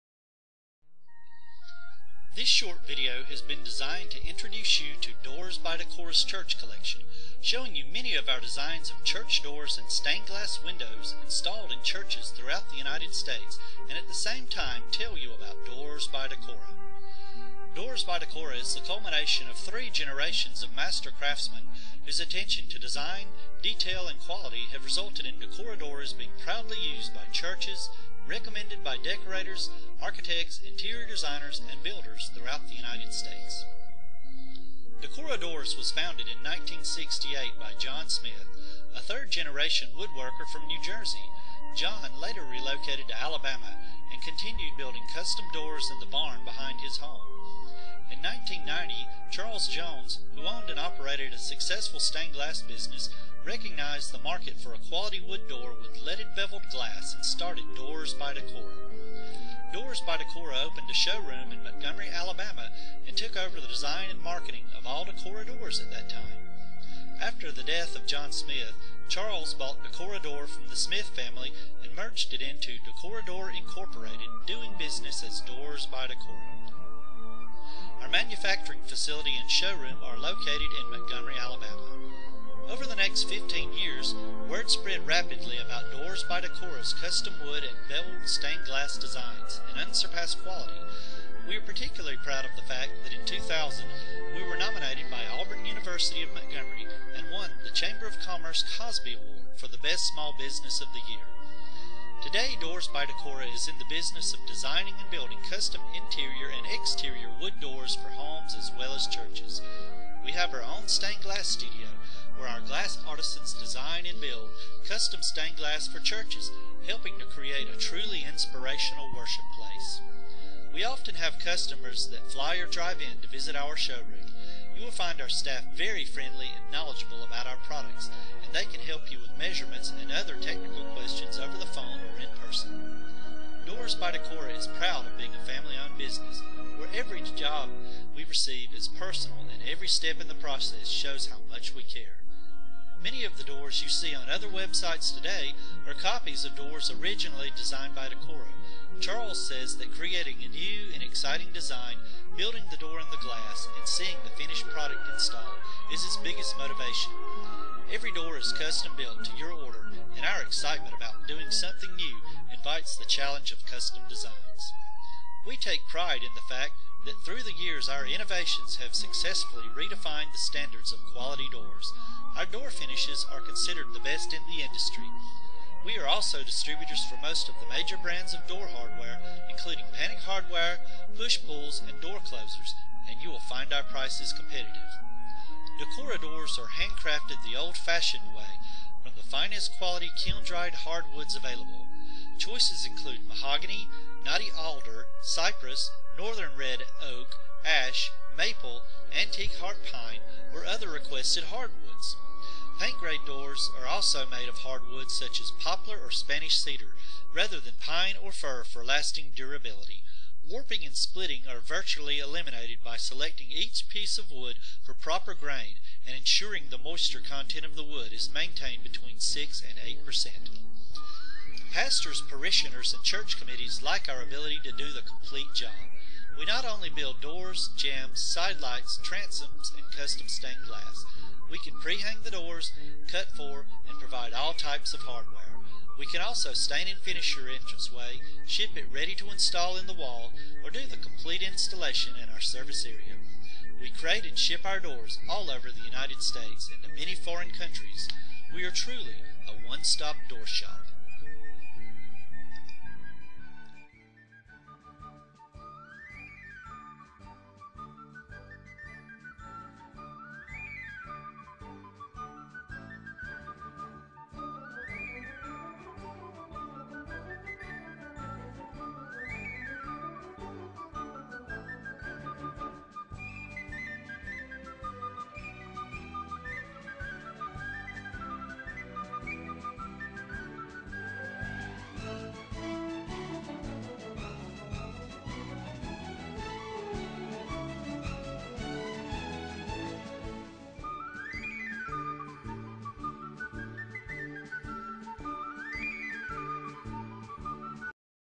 Doors By Decora Church Collection Introductry Video
Introduction to the Doors By Decora Church Door Collection.  Doors By Decora is a leading Church Door Manufacturer of Custom Church Doors and Church Stained Glass Windows.  This video shows a small collection of our many Custom Church Doors and Stained Glass Window designs.